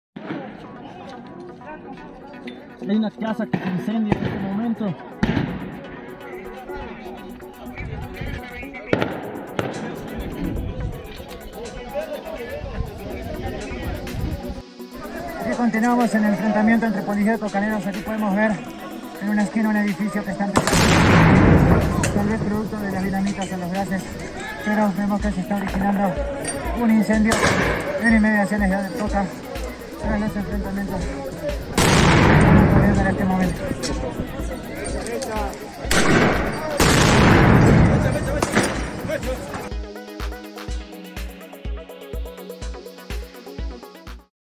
Videos 169 Facebook 16
Un numeroso contingente de uniformados de la Unidad Táctica de Operaciones Policiales (Utop) responde con gases lacrimógenos a las piedras y explosivos que usan los manifestantes.